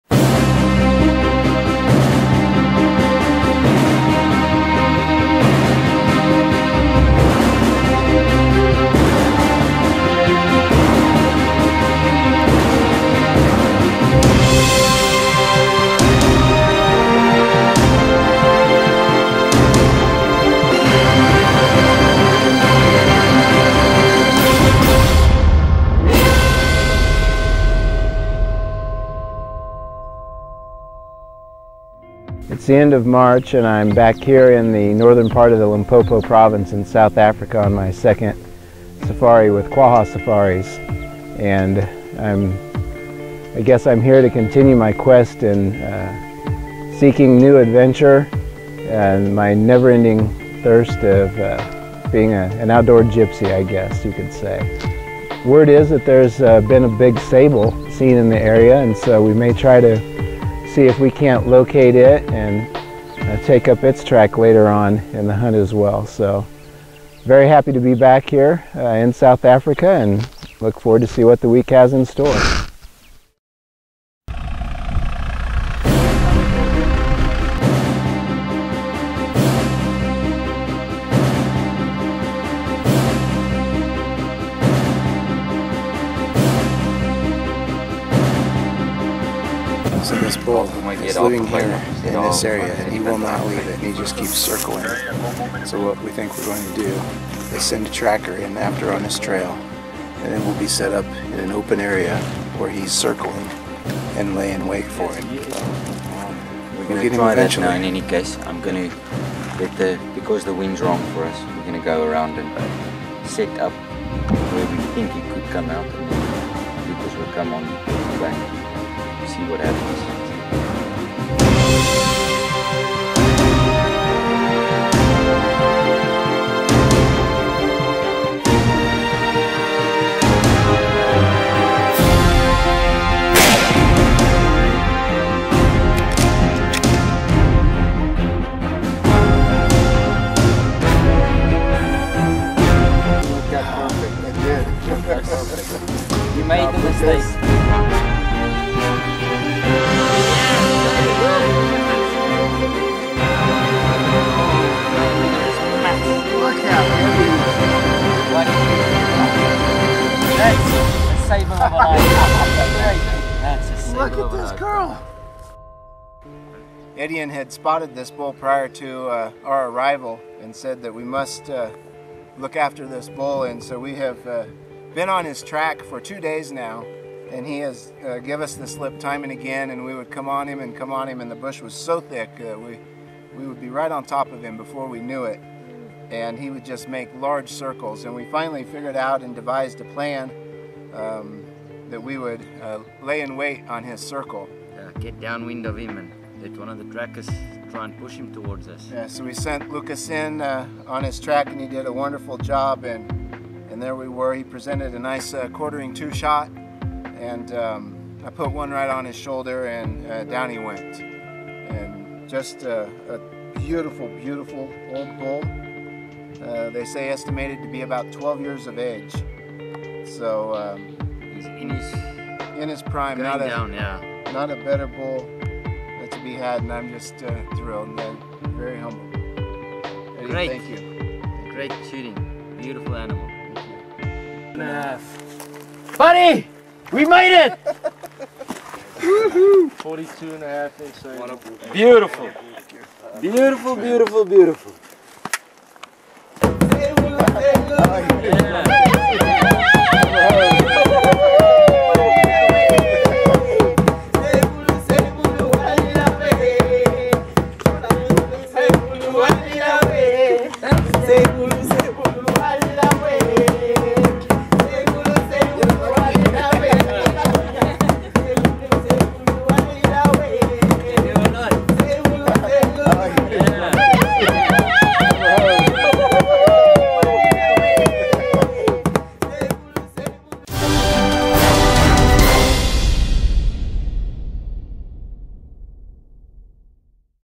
African Safari 2015 - Sable Hunt - Hunt to Table
Join Outdoor Gypsy and Hunter  Dana Peterson on his next hunting adventure as he tracks across the Limpopo province of South Africa in search of a SCI Gold Record Sable.  Hunting with Quagga Safaris with PH Ettiene.